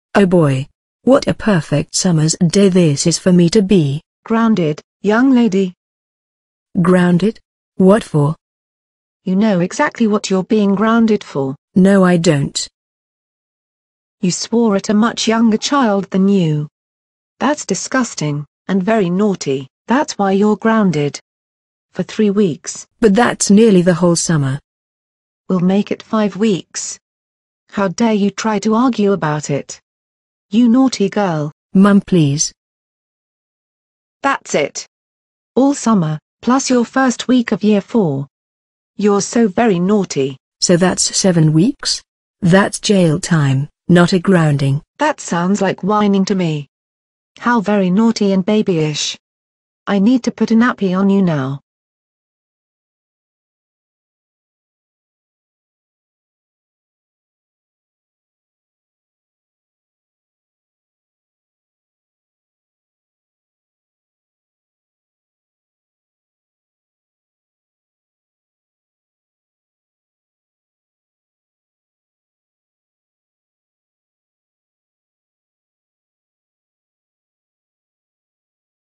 Episode 32: Whining About Being Grounded
Lily gets grounded and whines about it